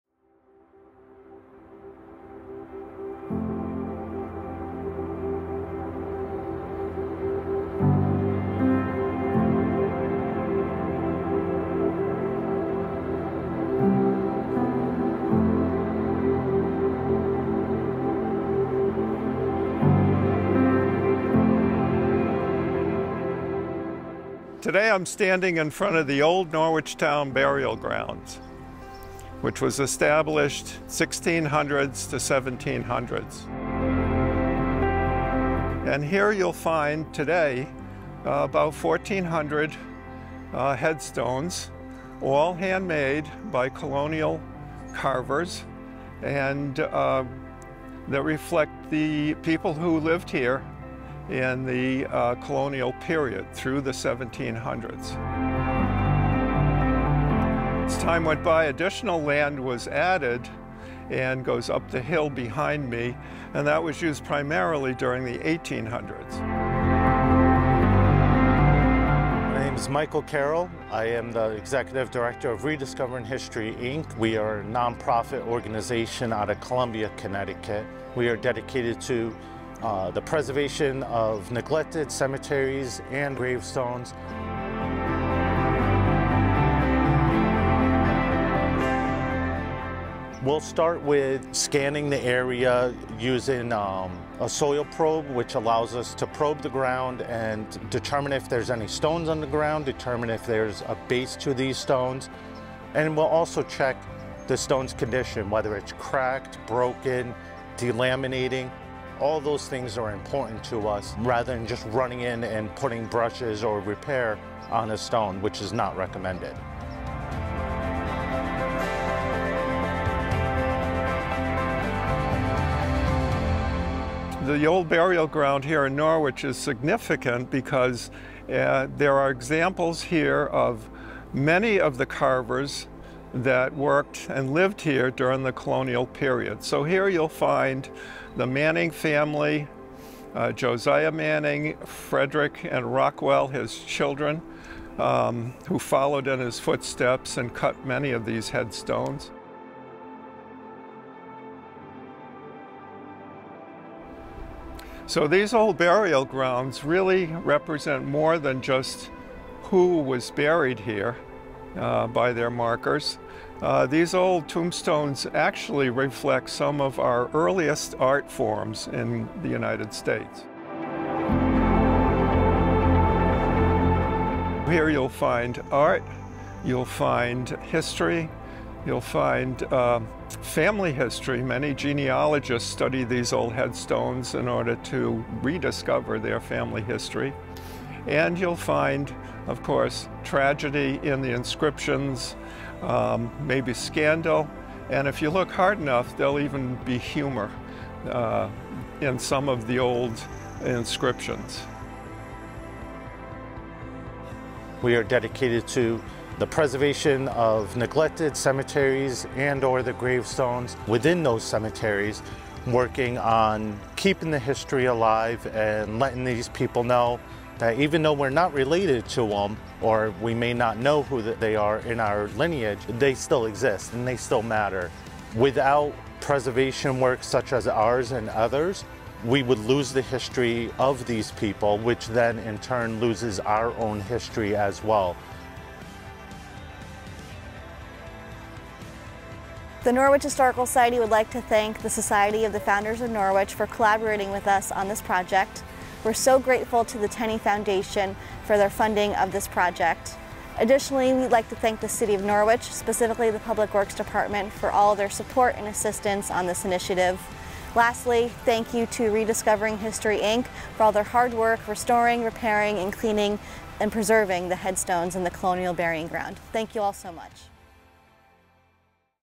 Restoring the Norwichtown Colonial Burying Ground
The Norwich Historical Society (NHS) and the Society of the Founders of Norwich (SFN) collaborated with Rediscovering History Inc. and the City of Norwich to restore headstones in the Norwichtown Colonial Burying Ground. Thanks to a generous grant from the Tenney Foundation, NHS and SFN were able to contract Rediscovering History Inc. to perform this important preservation work. We are so grateful to the Tenney Foundation for their support. We’d like to thank the City of Norwich and the Public Works Department for their assistance and help with this project.

    Established on January 4, 1700, the Norwichtown Colonial Burying Ground located off Town Street and Old Cemetery Lane is the second oldest burying ground in Norwich, Connecticut. The  land was sold by Lt. Samuel Huntington to be used for public burials. The burying ground is divided into two general areas, the first of which feature 17th and 18th century headstones; this section is located through the gates on Old Cemetery Lane.

     The second section is located to the north of the original section after crossing a small  footbridge. It runs up hill and ends at the Hubbard Gates on East Town Street. Several thousand  burials have been recorded here but only about 1300 headstones remain today. Free blacks and enslaved individuals were buried at the rear of the burying ground. Gov. Samuel Huntington, Hannah Arnold, Benedict Arnold’s mother, Boston Trowtrow, Black Governor of Norwich, and other prominent Norwich citizens are buried here.

Thank you to Barnberry Productions, LLC for creating the video. To view this video with captions, click on the box with letters CC from the onscreen menu.